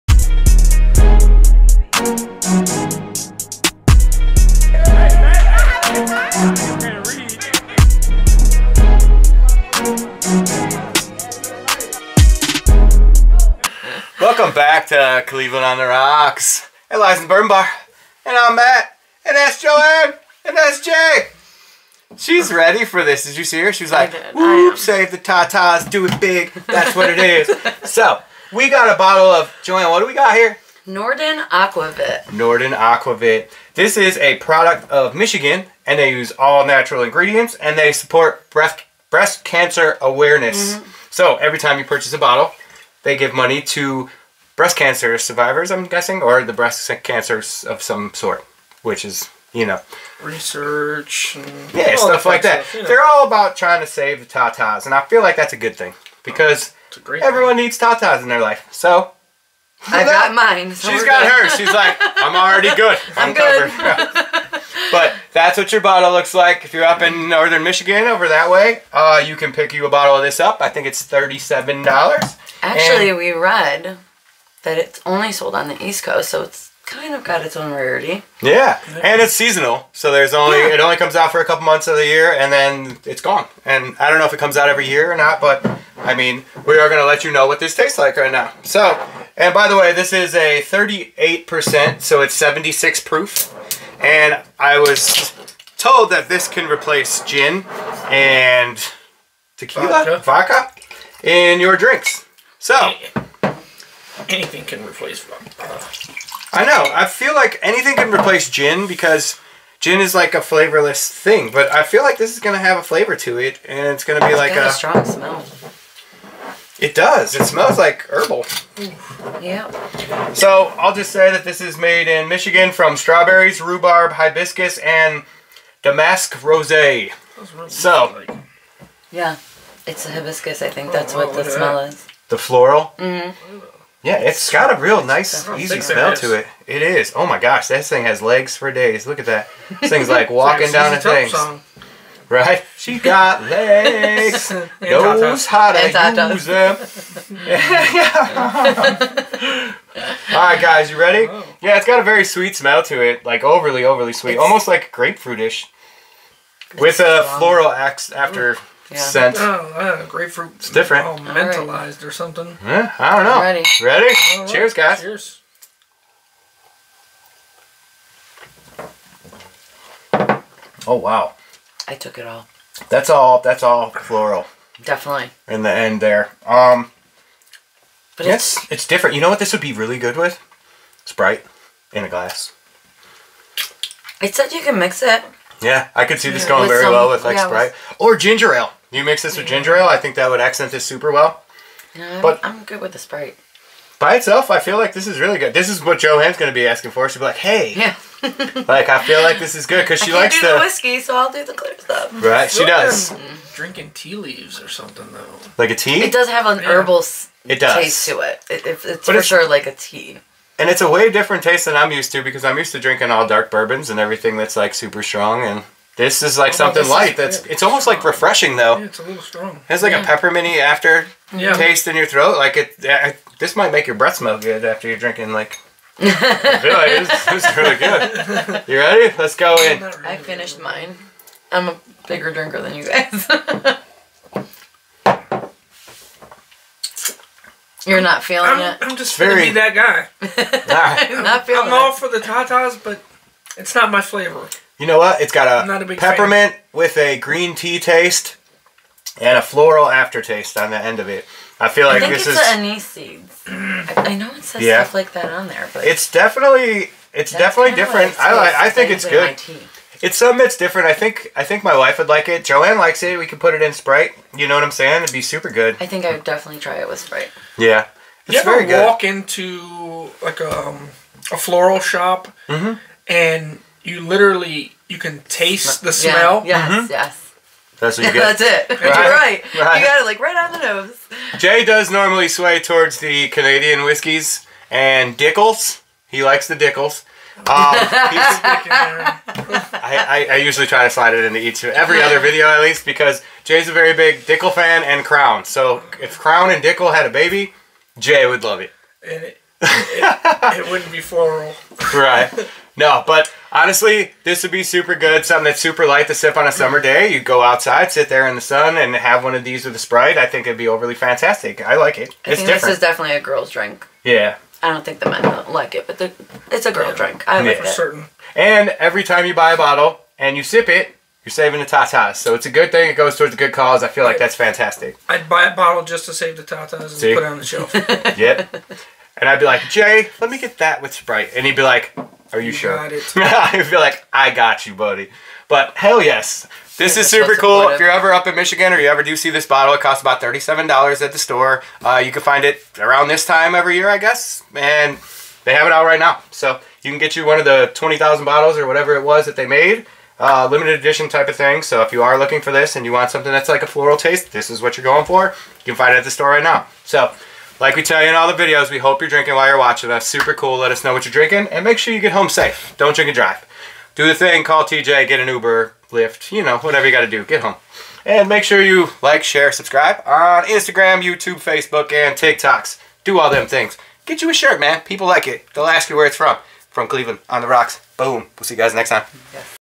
EP: 198 Norden Aquavit
it's Thursday. Help Us save the TaTa's and get you a bottle of Norden Aquavit. so every time a bottle is purchased they give some back to breast cancer research. It's a floral tea type of flavor that you didn't know you needed. have a great Thursday.

All of the shirts are 5 dollars off for a limited time

Links to the Shirts and Boozy Bag...

We appreciate every one of you and look forward to you sending in pictures of your shirts and bags.

Hope everyone has a great Thursday Cheers!